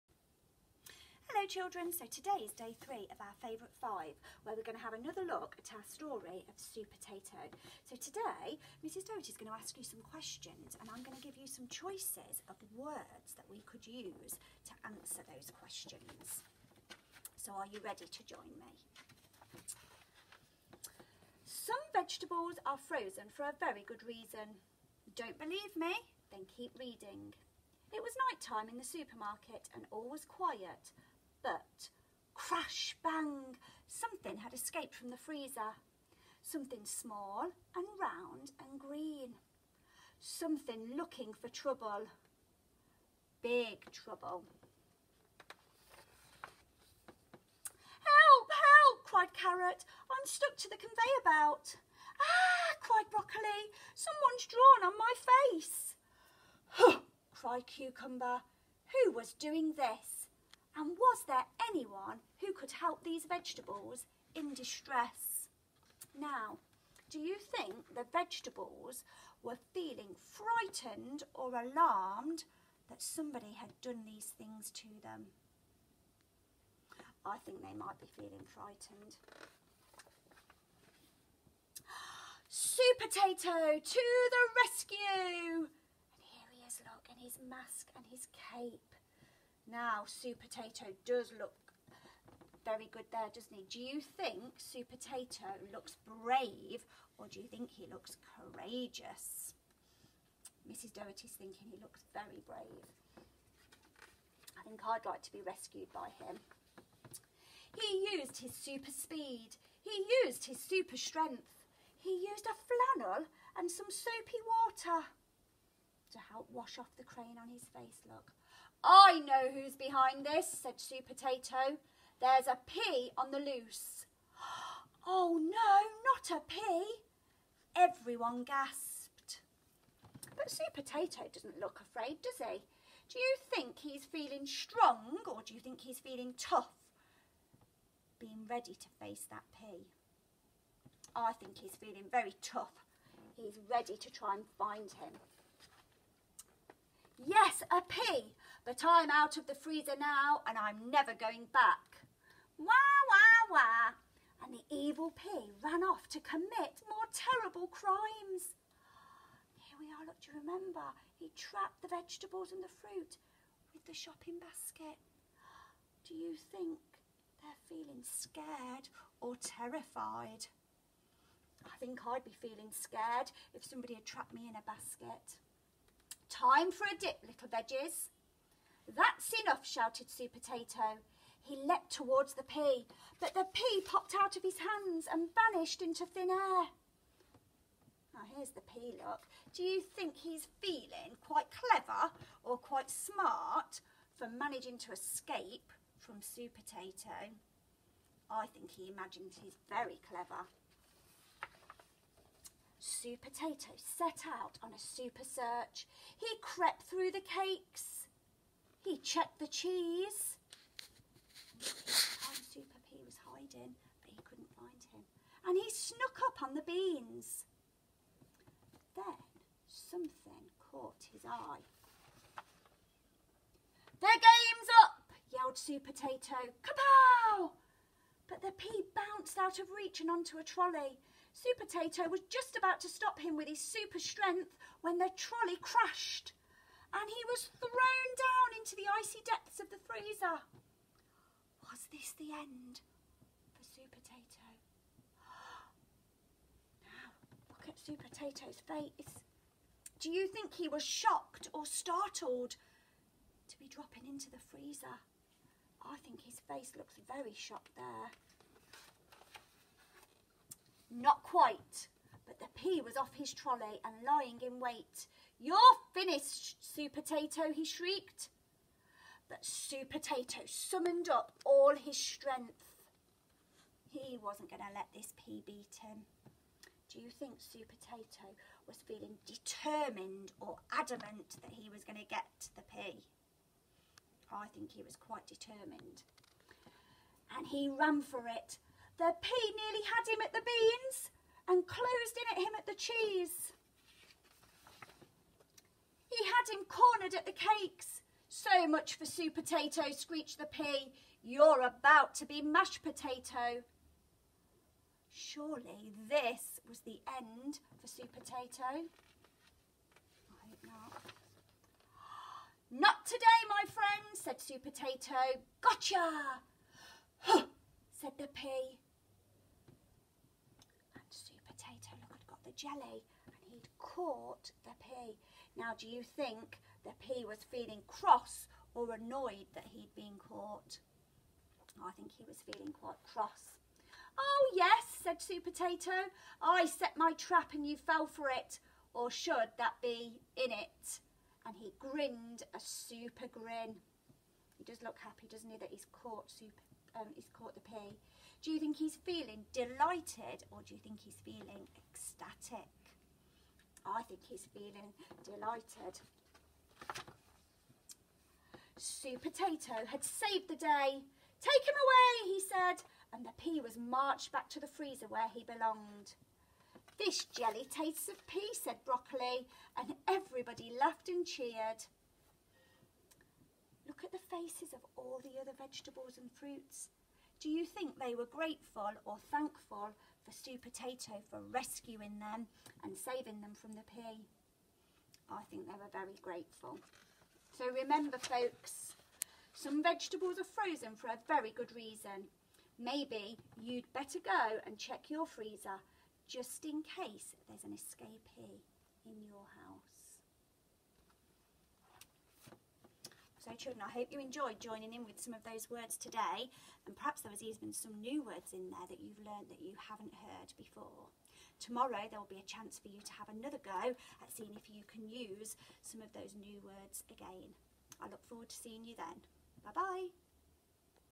Favourite 5 - Supertato Day 3
Help Mrs Doherty choose some exciting vocabulary to answer questions in the story.